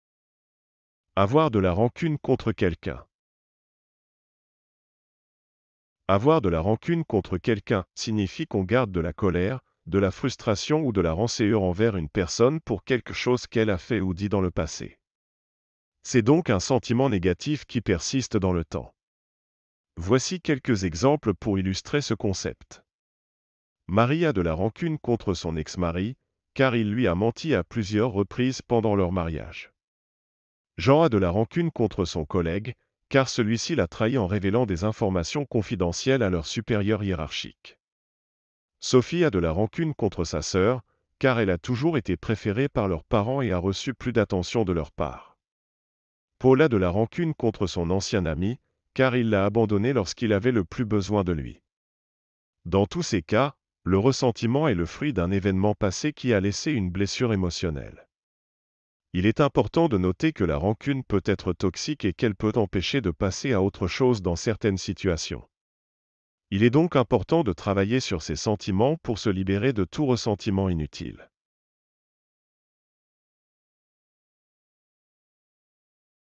Avoir de la rancune contre quelqu'un
"Avoir de la rancune contre quelqu'un" signifie qu'on garde de la colère, de la frustration ou de la rancœur envers une personne pour quelque chose qu'elle a fait ou dit dans le passé. C'est donc un sentiment négatif qui persiste dans le temps.
Voici quelques exemples pour illustrer ce concept :
- Marie a de la rancune contre son ex-mari, car il lui a menti à plusieurs reprises pendant leur mariage.
- Jean a de la rancune contre son collègue, car celui-ci l'a trahi en révélant des informations confidentielles à leur supérieur hiérarchique.
- Sophie a de la rancune contre sa sœur, car elle a toujours été préférée par leurs parents et a reçu plus d'attention de leur part.
- Paul a de la rancune contre son ancien ami, car il l'a abandonné lorsqu'il avait le plus besoin de lui.
Dans tous ces cas, le ressentiment est le fruit d'un événement passé qui a laissé une blessure émotionnelle. Il est important de noter que la rancune peut être toxique et qu'elle peut empêcher de passer à autre chose dans certaines situations. Il est donc important de travailler sur ses sentiments pour se libérer de tout ressentiment inutile.